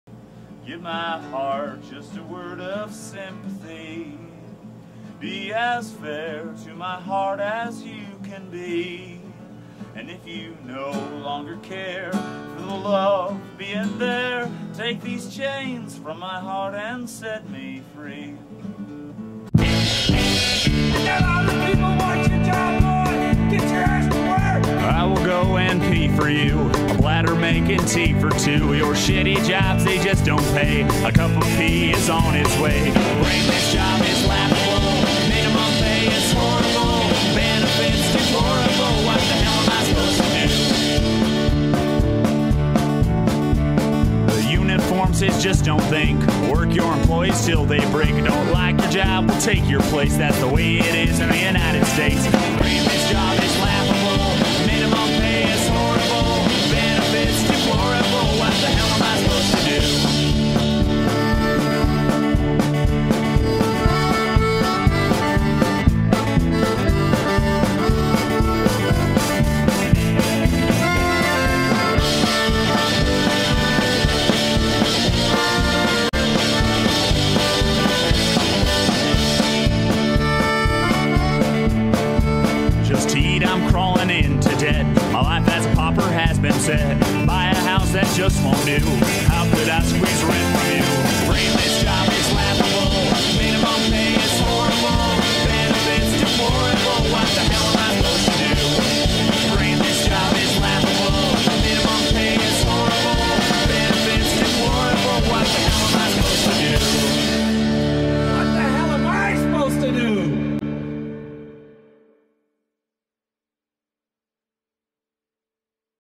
Brainless Job is Laughable
Corporate hack got sick of the crap and wrote a song about the New American Life and Attitude.